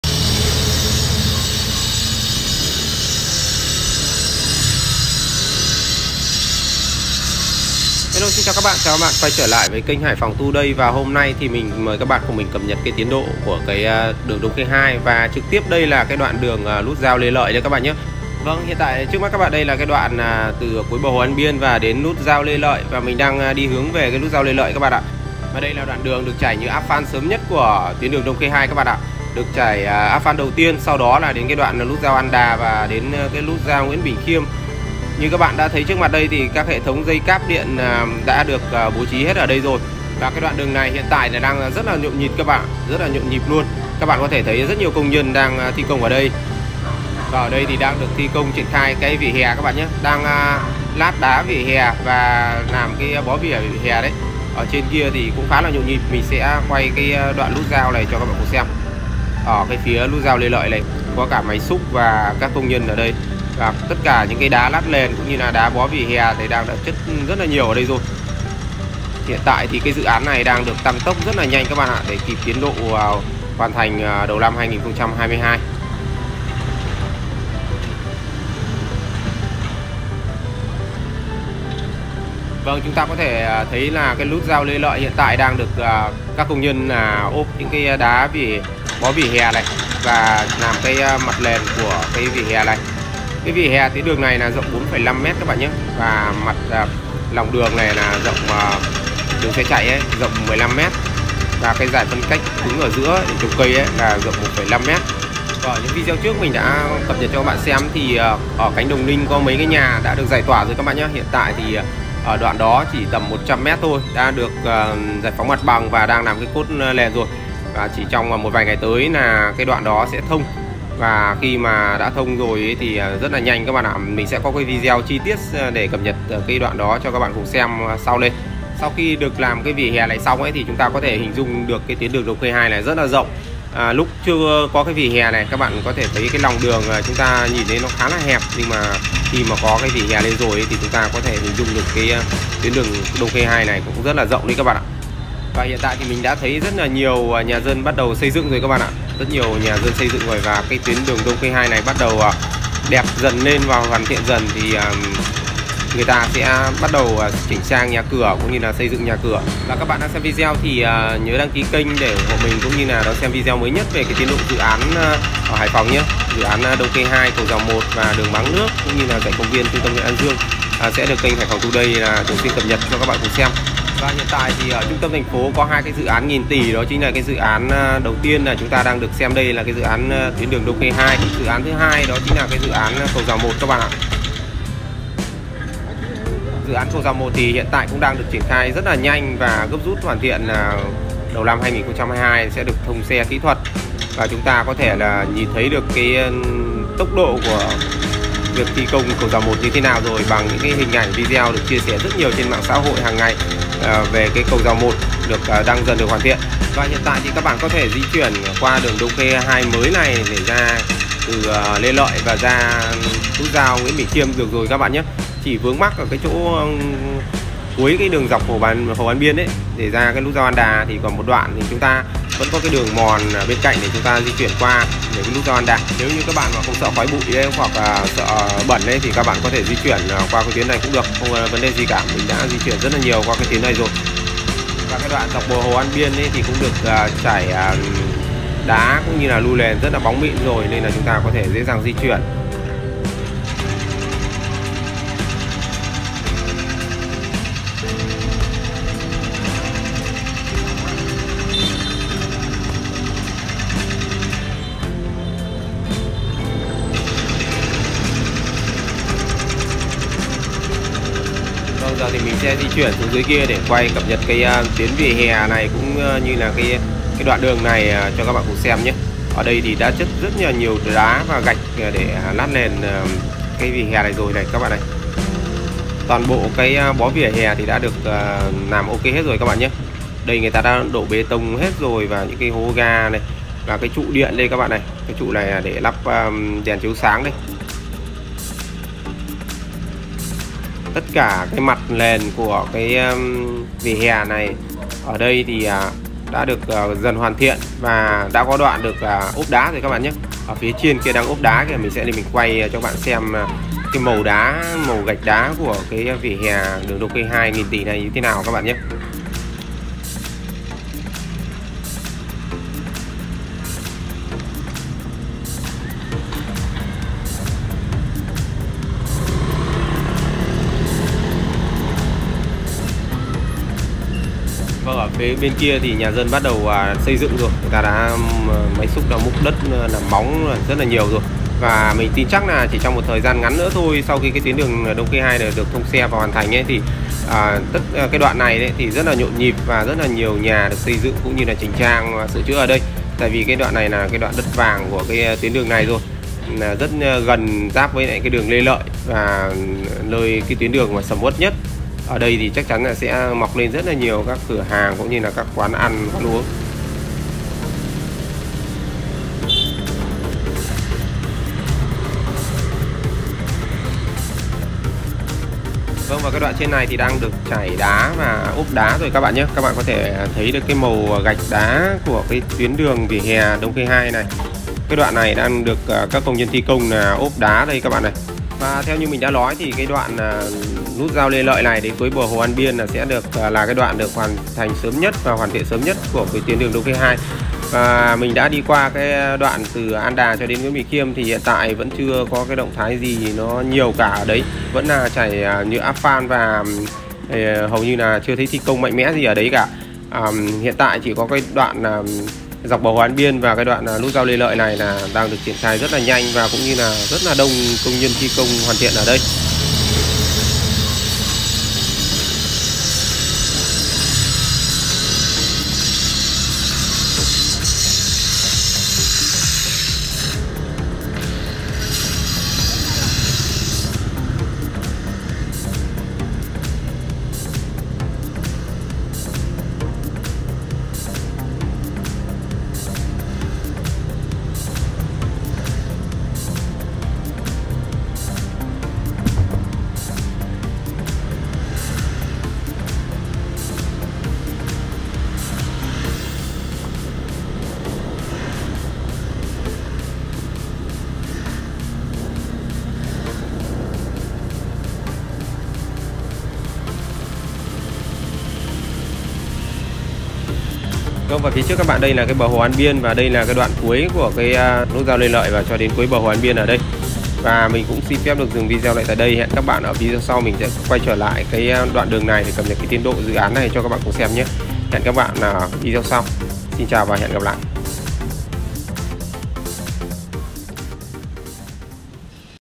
Lát Đá Vỉa Hè Dự Án Nghìn Tỷ Đường Đông Khê 2 🔴 Nút Giao Lê Lợi Đang Về Đích | Hải Phòng Today
• Xin gửi tới các bạn hình ảnh mới nhất dự án Đông Khê 2 , đoạn nút giao Lê Lợi đến hồ An Biên đã được lát đá vỉa hè ,mời các bạn xem video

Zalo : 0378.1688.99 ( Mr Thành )
© Bản quyền thuộc về @haiphongtoday
© Copyright by @haiphongtoday ☞ Do not Reup